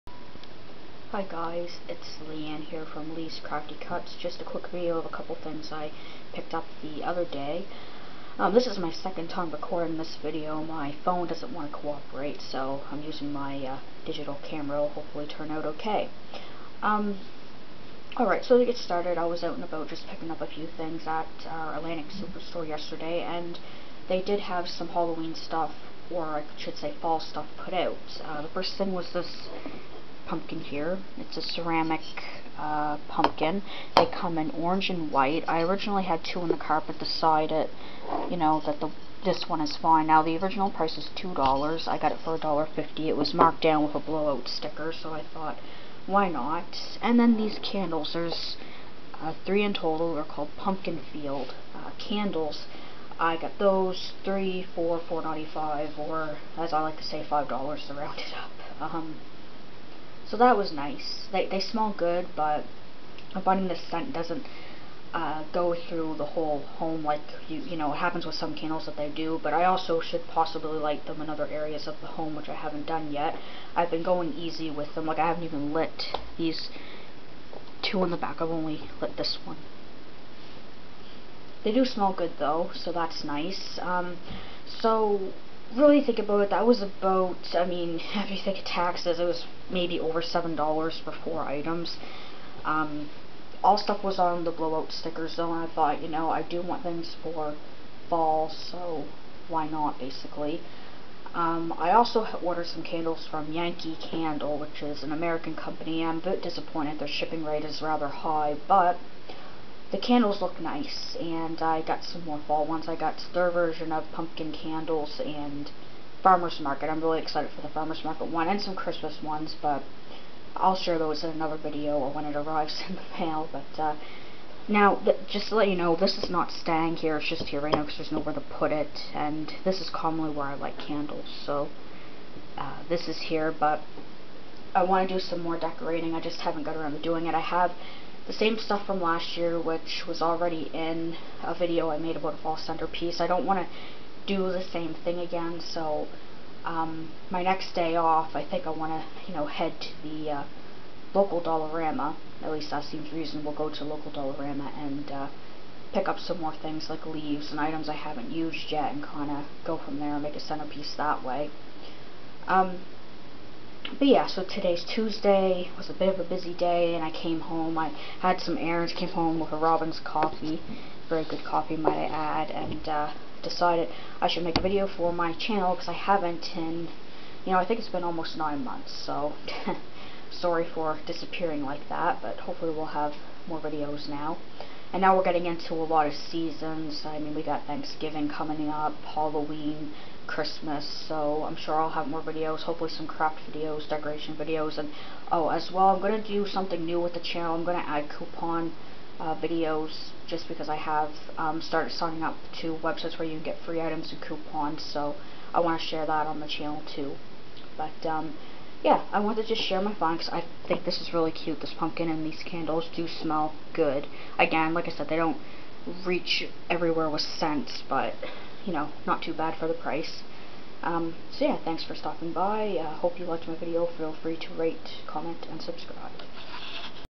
Some Fall Finds From Atlantic Superstore
Just a video of some Pumpkin Field candles and a ceramic pumpkin on sale at one of the grocery stores in my town.